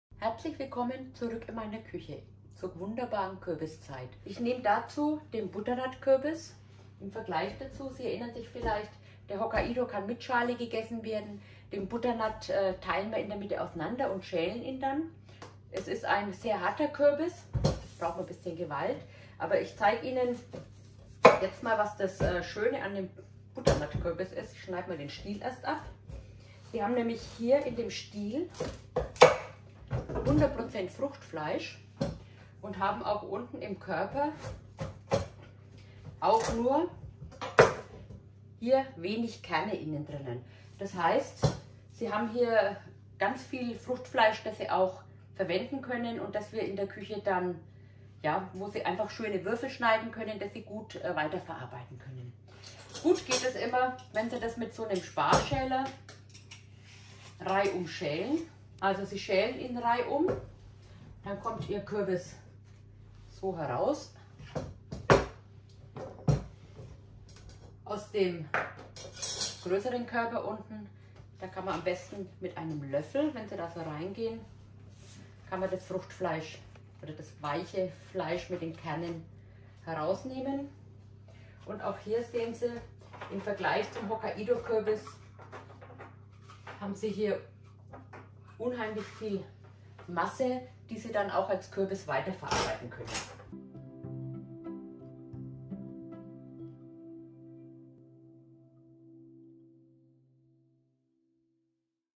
Tipp der Woche #9: Kürbiszeit. Hokkaido vs. Butternut mit Kreisbäuerin Renate Ixmeier. Bitte teilen!
Bitte hinterlasse ein Kommentar & Like, wenn Dir das Video gefallen hat.